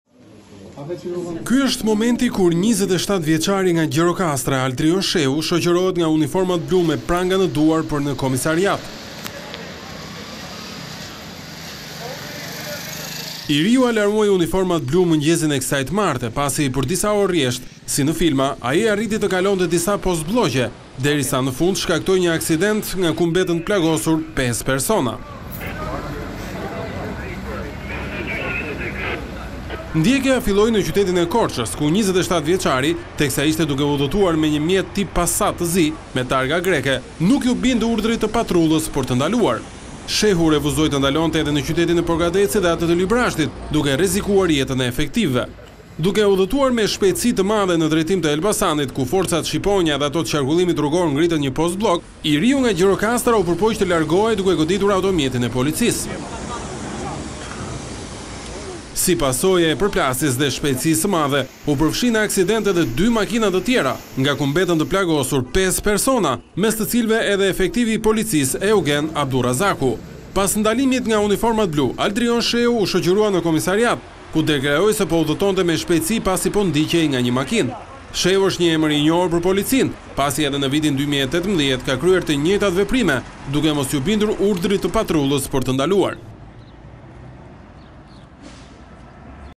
Ndjekje si në në filma/ 5 të plagosur, mes tyre 1 polic - News, Lajme - Vizion Plus
Dalin emrat e pesë personave që mbetën të plagosur në ngjarjen me sfond kriminal në Krastë të Elbasanit. Një makinë që ndiqej nga policia që nga qyteti i Korçës ka tentuar të çajë postbllokun e ngritur enkas për ndalimin e këtij mjeti në Krastë të Elbasanit. Në aksident është përfshirë dhe një autoshkollë, e cila dyshohet se ka qenë rastësisht aty dhe nuk ka pasur lidhje me ngjarjen.Nga përplasja e të paktën 4 mjeteve, mbeti i plagosur ekektivi i policisë Eugen Abdurrazaku, si dhe shtetasit Diana Plaka, Durim Plaka (burrë e grua), Albert Cani (instruktor autoshkolle) dhe Adriana Mukja.Dyshohet se në këtë ngjarje me prapavijë kriminale, të ketë qenë dhe një mjet tjetër i përfshirë, që është larguar me shpejtësi nga vendngjarja. Pikërisht ky mjet dyshohet të ketë plagosur dhe punonjësin e policisë. Raportimet e para bënë të ditur se në vendngjarje ka pasur edhe të shtëna armësh, mirëpo ende nuk ka një konfirmim zyrtar nga Policia e Shtetit në lidhje me dinamikën e të gjithë kësaj situate.